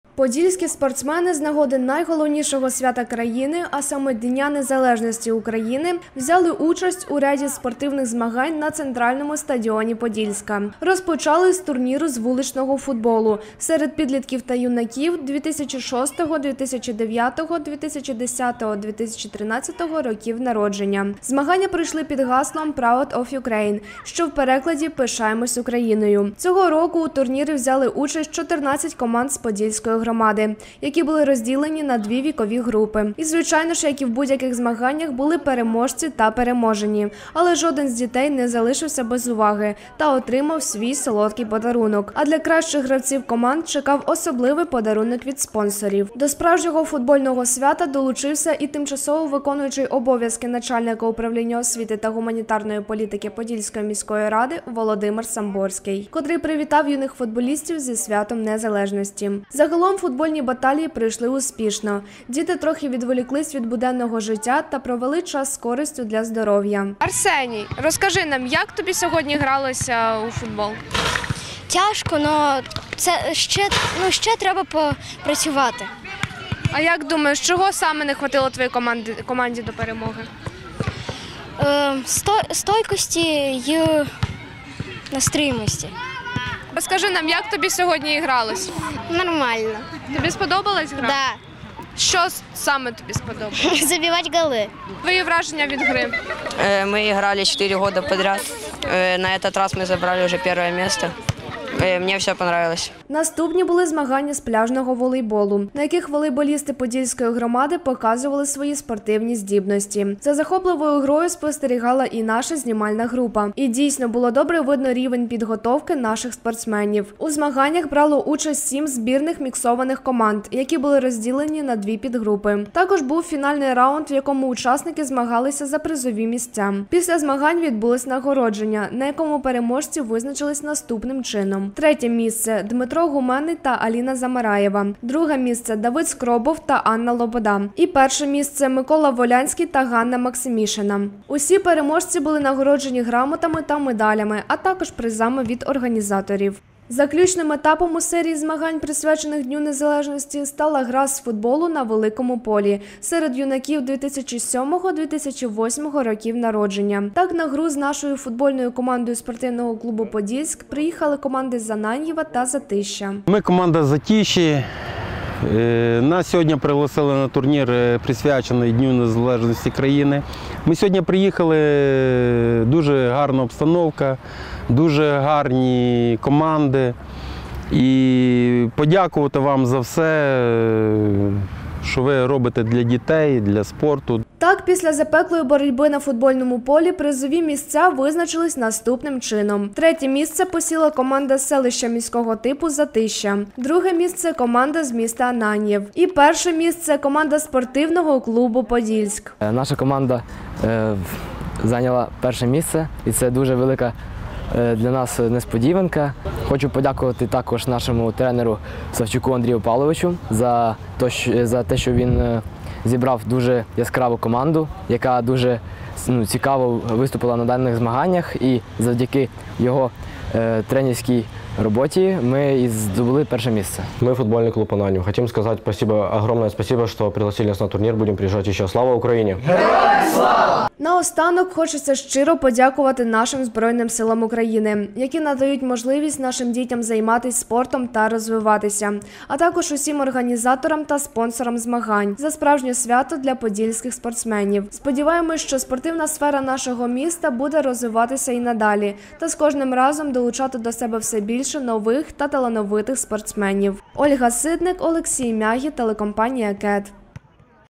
Низка спортивних змагань до Дня Незалежності України
З нагоди Дня Незалежності України на центральному стадіоні Подільська відбулася низка спортивних змагань.